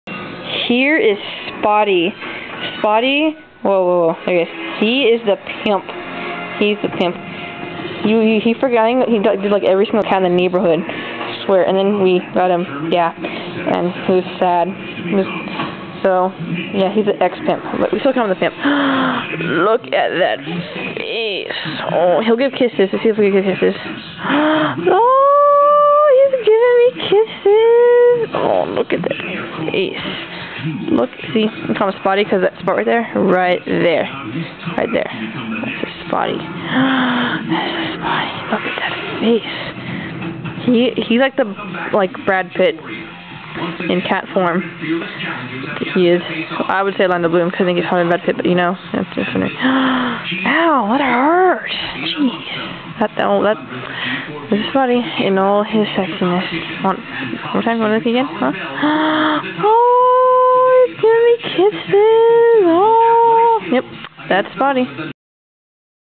Spotty the pimp
Spotty the ex pimp cause he got neutered lol